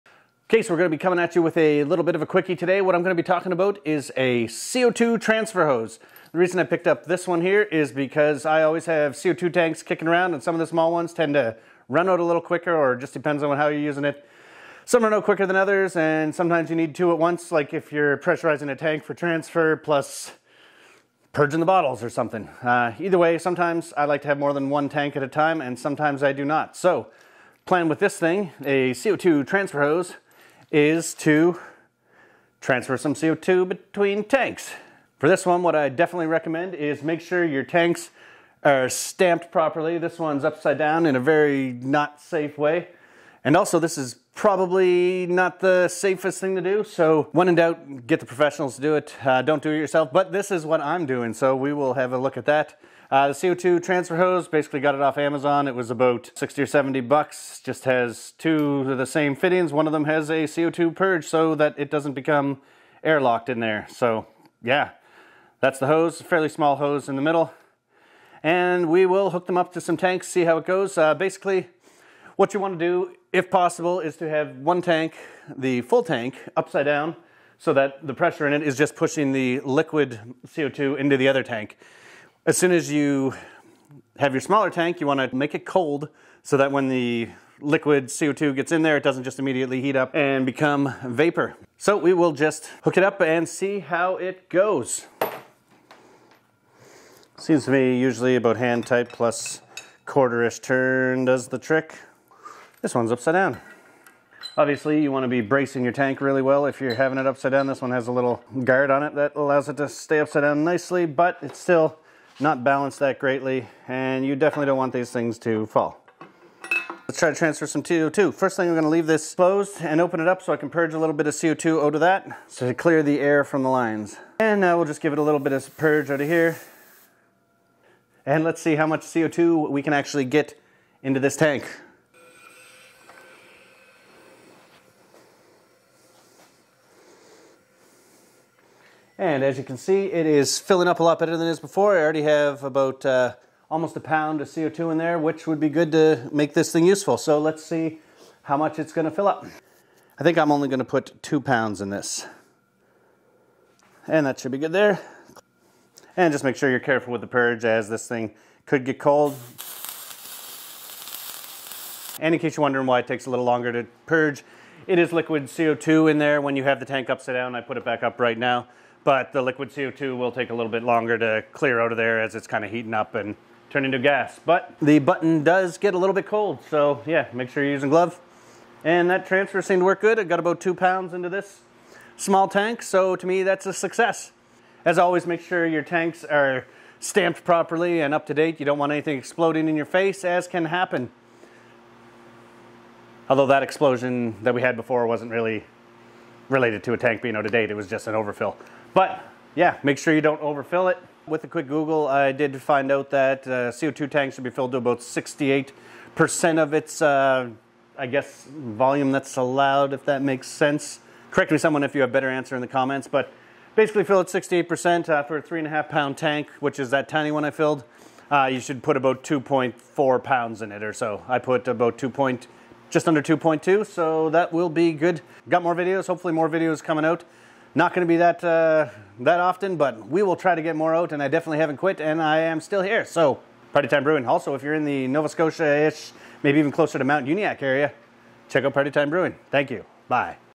Easy and Cheap CO2 - Bottle to Bottle Transfer
Here's how I use a CO2 transfer hose to minimize the number of trips and cost of filling CO2 tanks. Make sure you don't overfill!

Do not try this at home. Tanks are under pressure and can really hurt/kill you if something goes wrong. Make sure all your tanks have been visually and hydro tested and are braced well prior to filling. A young firefighter was recently killed around here when an improperly inspected CO2 tank exploded. Take this seriously!

That's the most serious thing I've ever written. But if you are thinking of risking it, here's a link to the CO2 transfer hose: